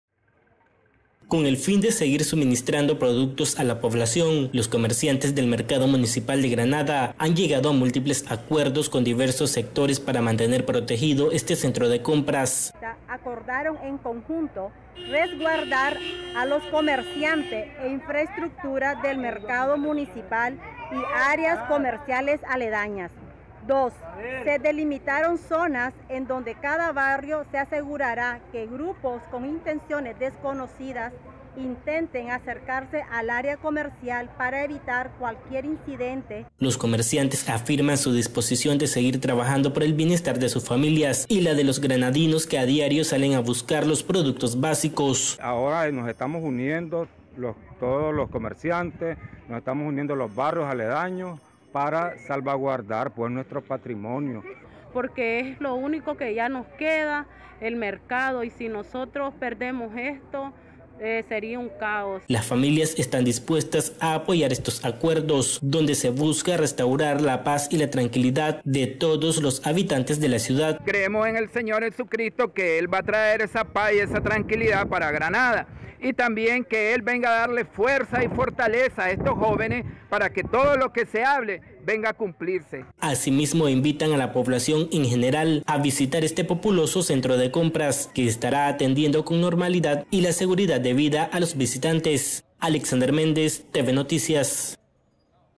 EL MERCADO DE GRANADA PERMANECERÁ RESGUARDADO POR LA POBLACIÓN
Con el fin de seguir suministrando productos a la población, los comerciantes del mercado municipal de Granada han llegado a múltiples acuerdos con diversos sectores para mantener protegido este centro de compras.